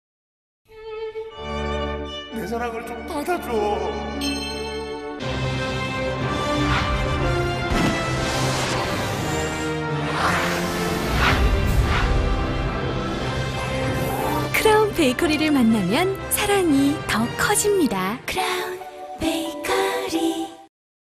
이승기 출연 크라운베이커리 CF - 사랑이 더 커집니다 편 (20초 버전)
가수 이승기가 출연한 크라운베이커리 CF의 20초 버전. 크라운베이커리에서 케이크를 만들던 이승기가 연인의 사랑을 받아주기 위해 슈퍼맨으로 변신하여 케이크를 전달하는 모습을 통해 크라운베이커리를 만나면 사랑이 더 커진다는 내용을 담고 있음.